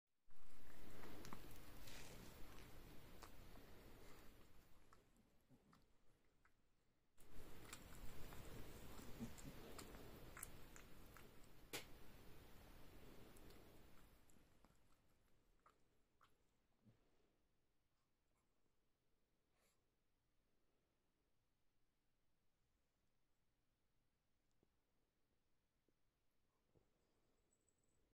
Taelyn grooming Tahlia
It is so cute when they groom each other.